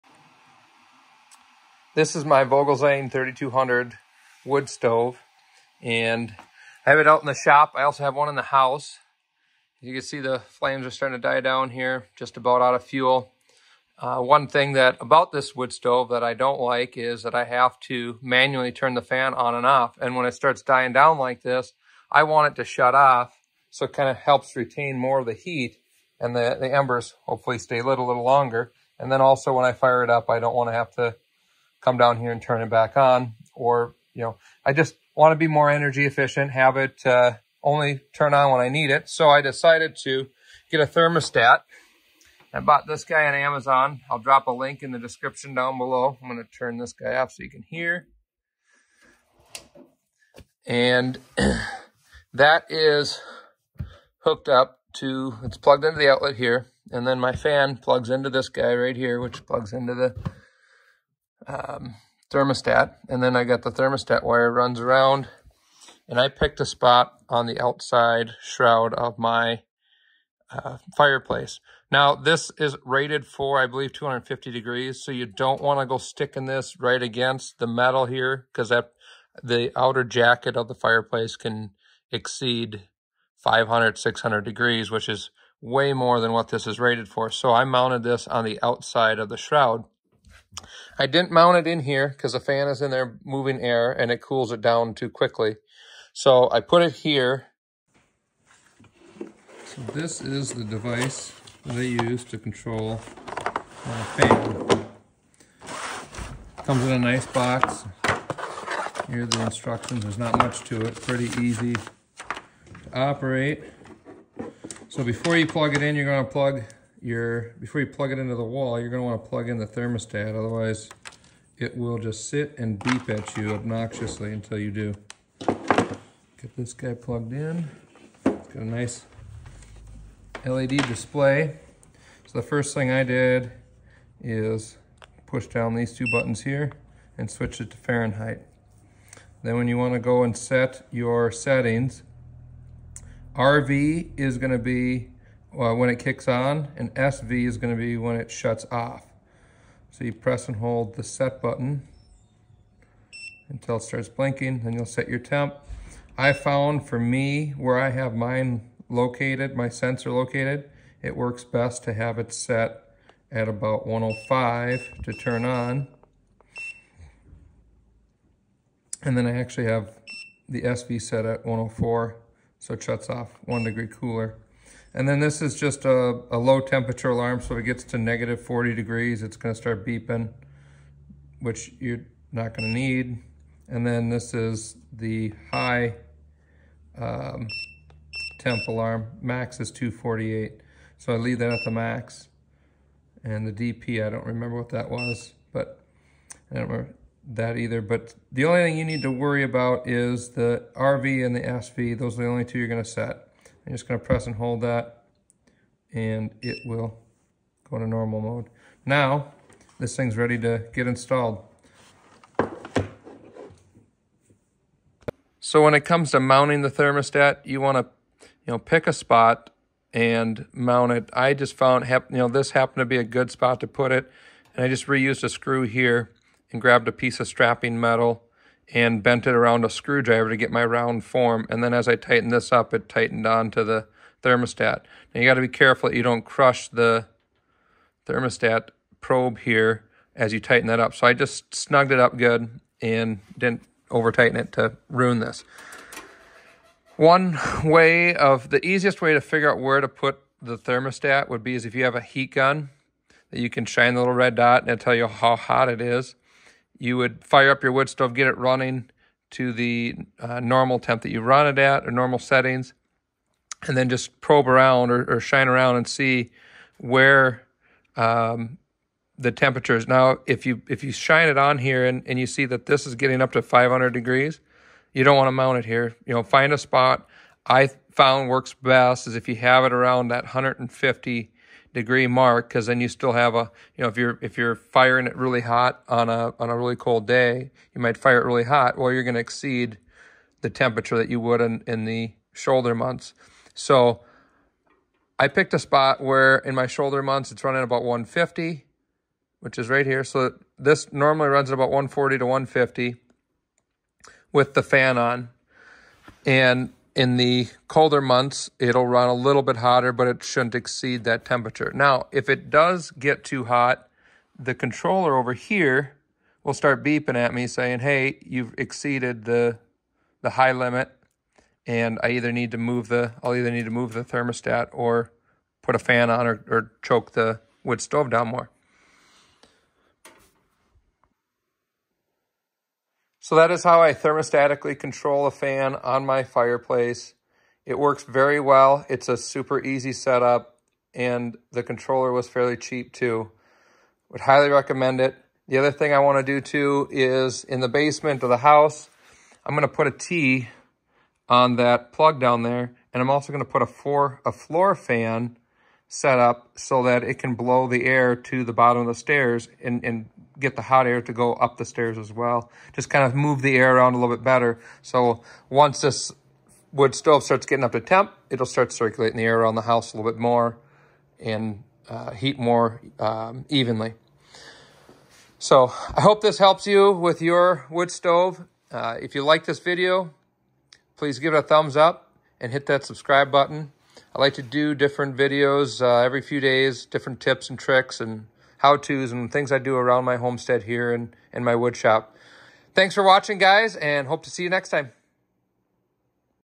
Wood stove auto fan on off switch install
How to add a thermostatically controlled switch to your wood stove fan.  This will cause the fan to turn on when the stove gets to temp and tell it to shut off once the wood stove starts to run out of fuel.

Thermostat switch: